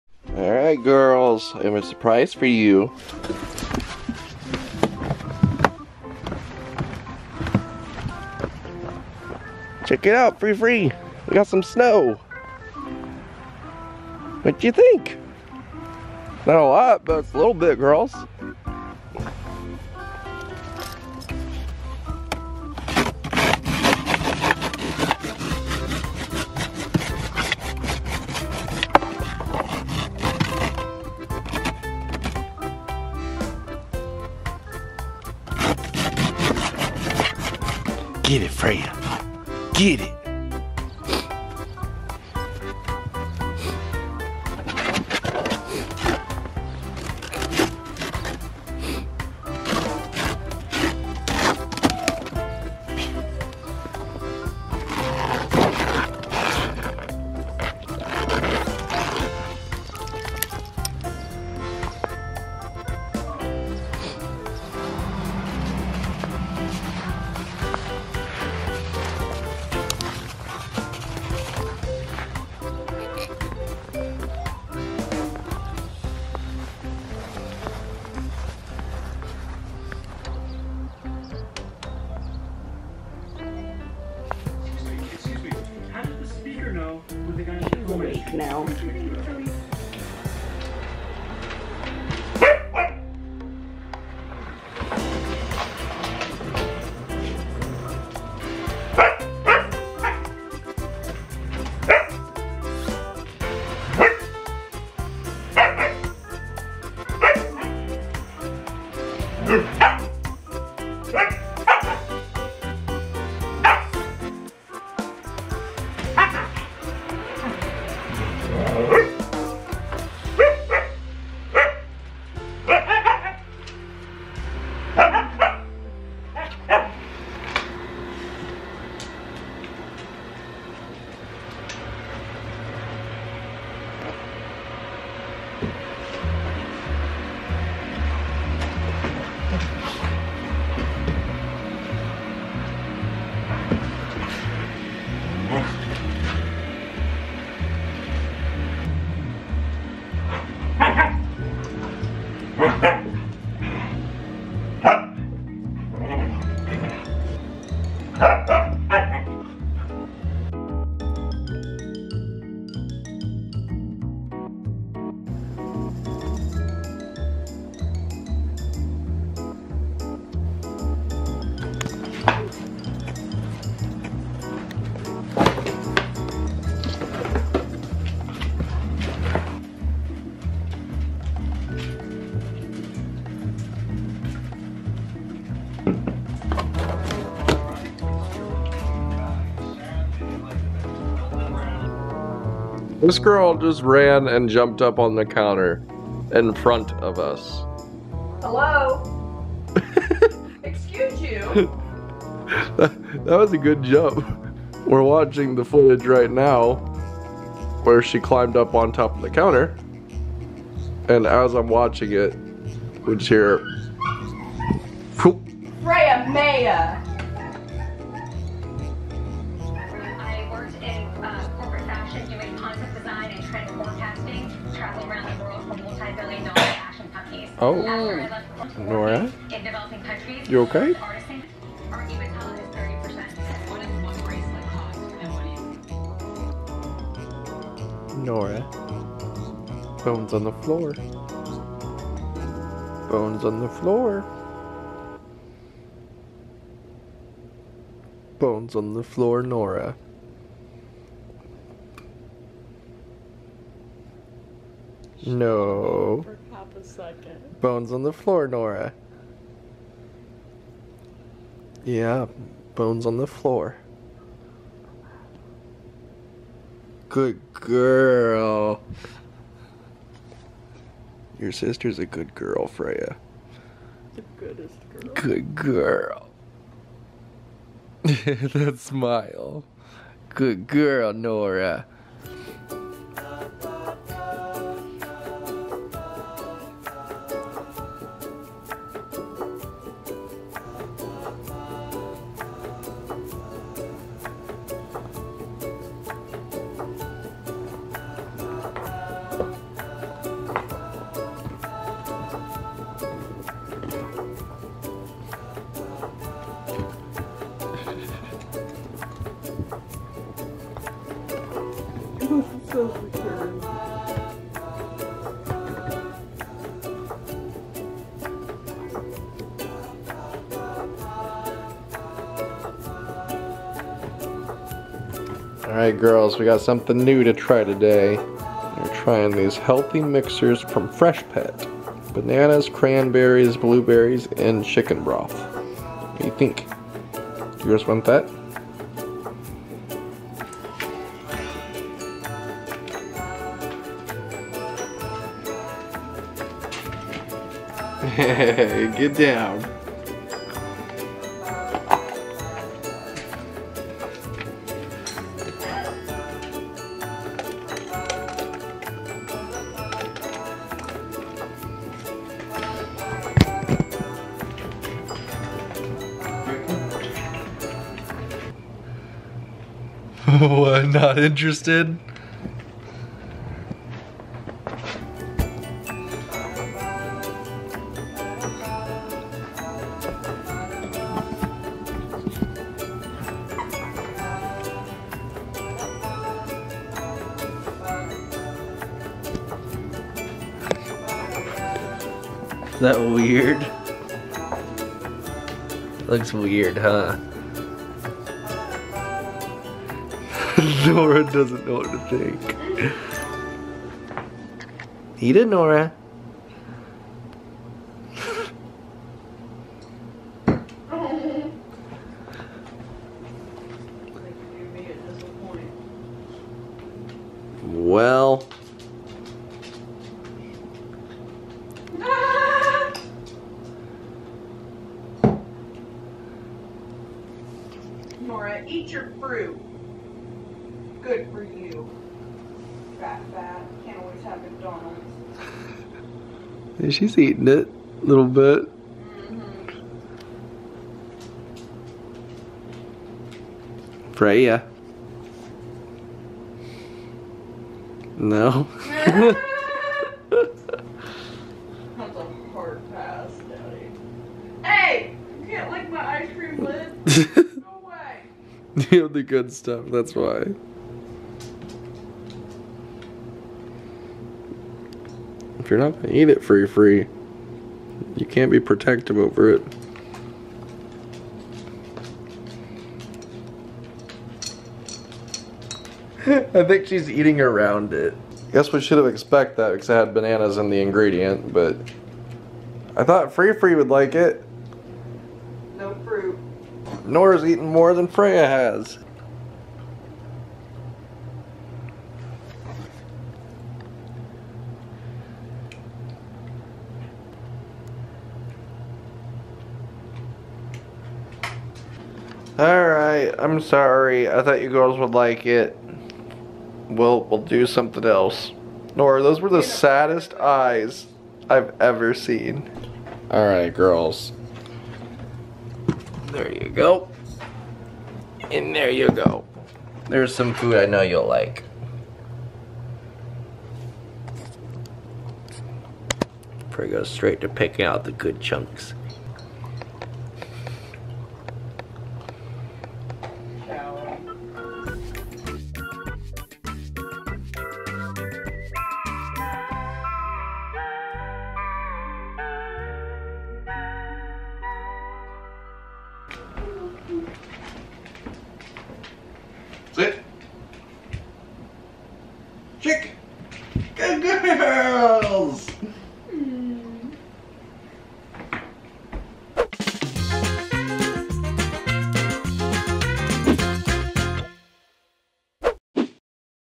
BONES ON THE FLOOR NORA!
We have some spoiled huskies but we try to enforce the "bones on the floor" rule. Nora wasn't too happy with me but she was still a good girl!

► Send Us Mail & We'll open it on Video!

Paw Record
Gas City Indiana
46933

Wishlist Items